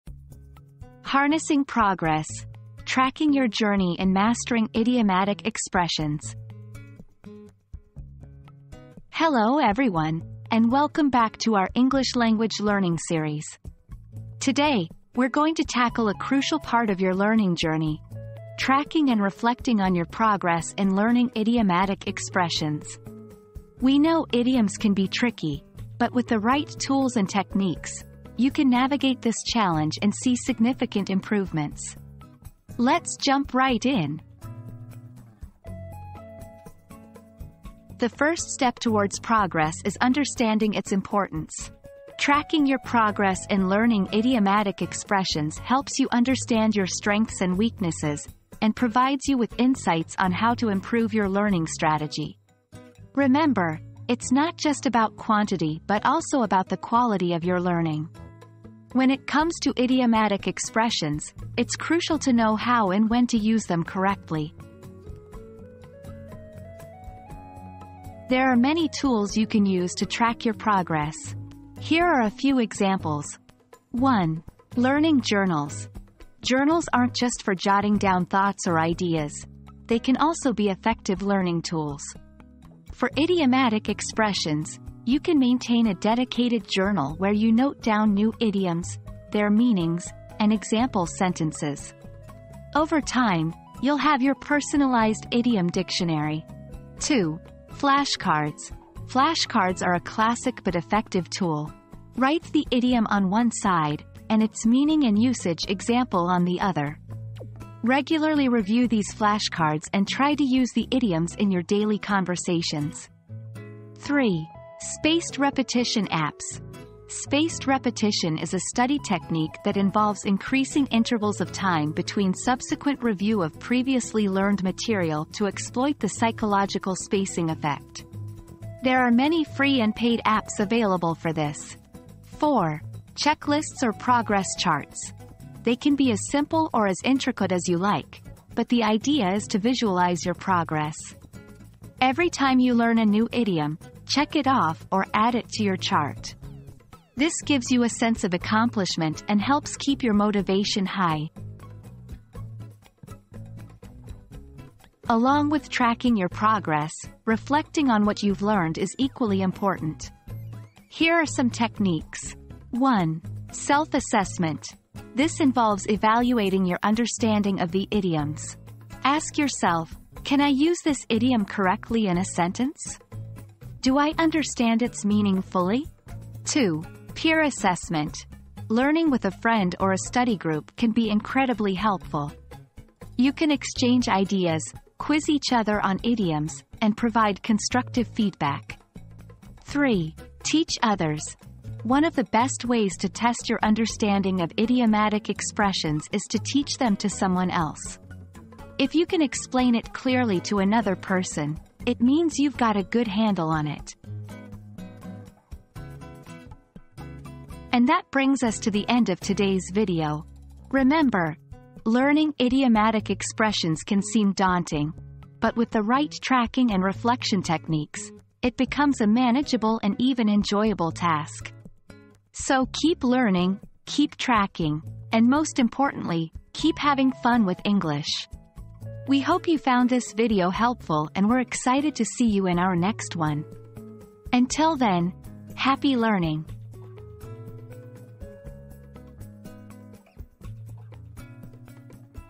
Harnessing Progress: Tracking Your Journey in Mastering Idiomatic Expressions
Mastering Idiomatic Expressions: Track Your Progress and Improve Your Skills • Join us in this English language learning video as we explore the importance of tracking your progress in mastering idiomatic expressions. Discover effective tools and techniques to improve your learning strategy and become fluent in using idioms correctly. Watch now and take your language skills to the next level!

00:00 • Introduction - Harnessing Progress: Tracking Your Journey in Mastering Idiomatic Expressions
00:37 • The Importance of Tracking Progress
01:07 • Tools for Tracking Progress
02:34 • Techniques for Reflecting on Progress